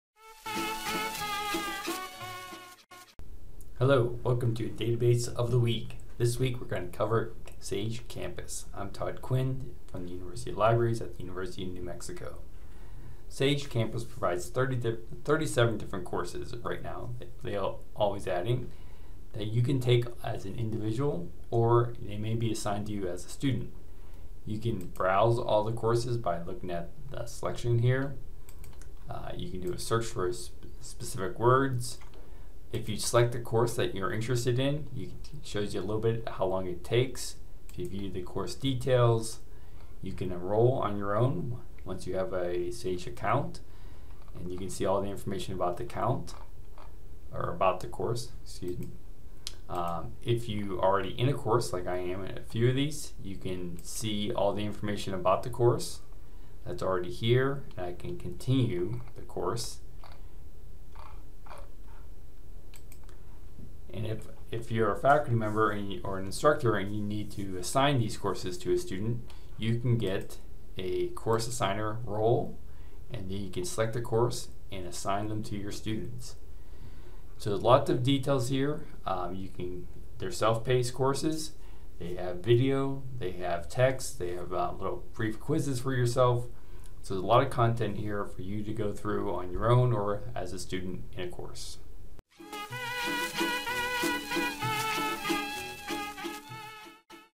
Overview of Sage Campus
Sage Campus is a database of 35+ courses and this video provide a brief overview